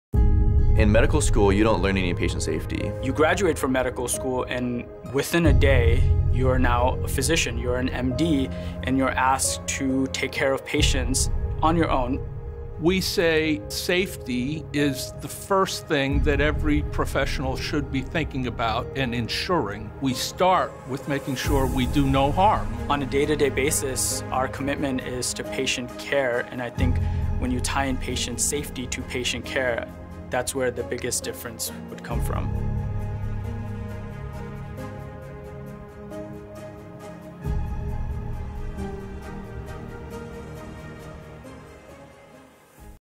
Patient Safety is Becoming Lesson No. 1.
Learn how to tie patient safety into healthcare professionals training with the APSS Blueprint, Patient Safety Curriculum.